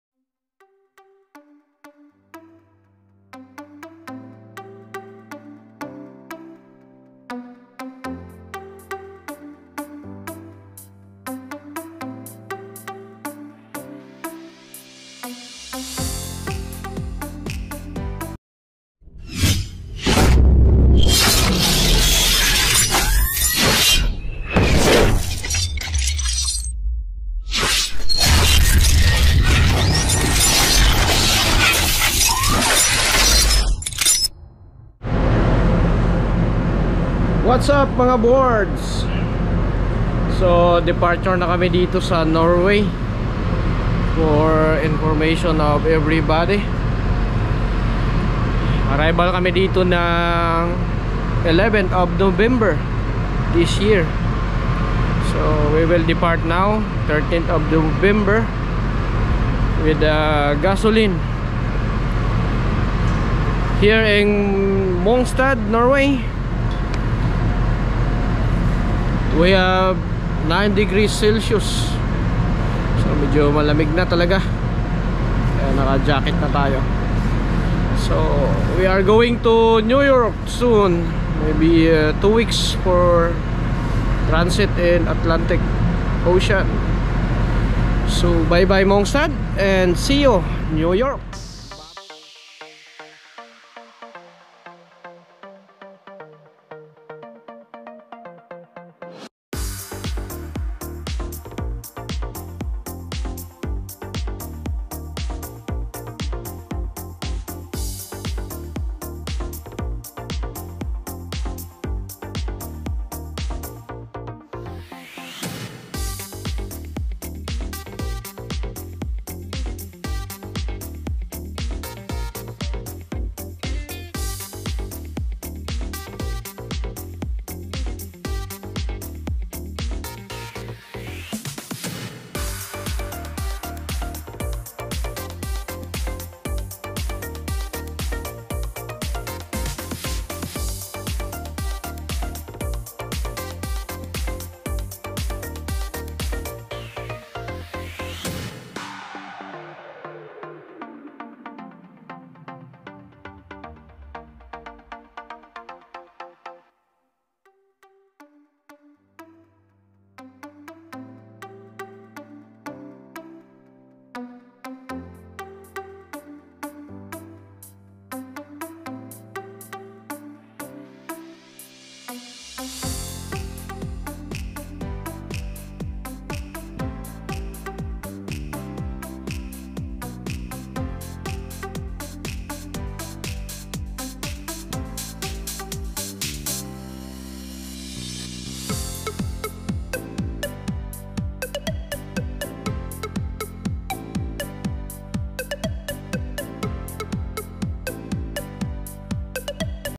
Departure in Mongstad,Norway going to New York USA#shorts
We depart here in Mongstad,Norway little bit cloudy and has 9°deg.temperature ice is coming next month. But in New york ports there is a big possibility that ice is already started in all areas in ports. See u soon New York USA..